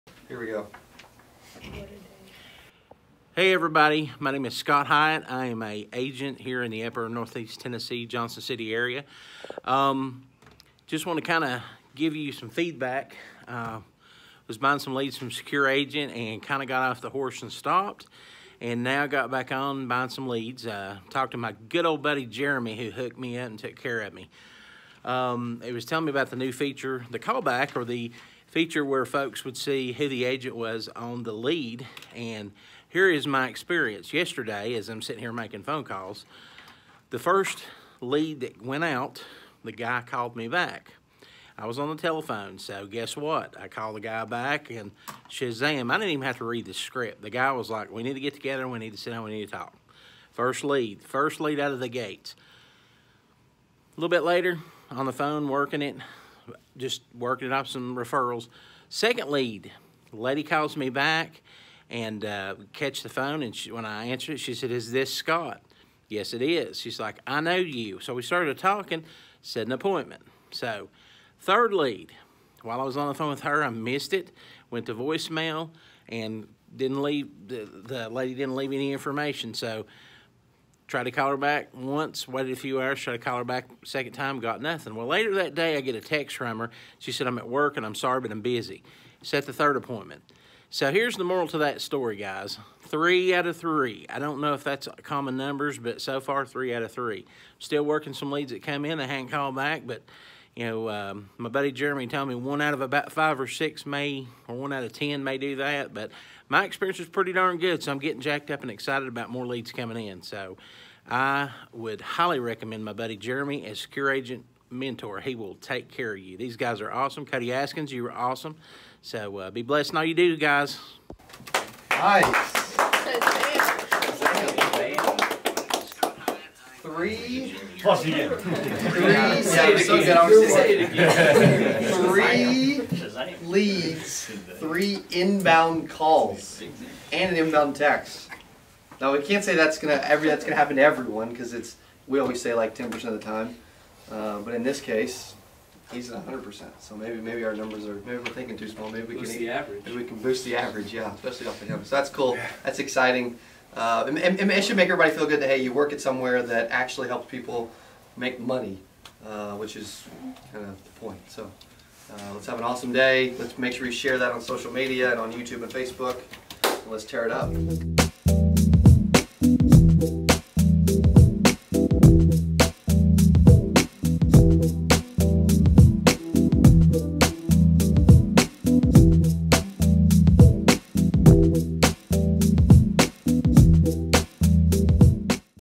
Insurance Agent Sets 3 Appointments with 3 Leads!
This agent set 3 appointments off his FIRST 3 leads! 🔥🔥

Thanks Scott Hyatt [Transamerica Network Agency] for the video testimonial and for doing business with us!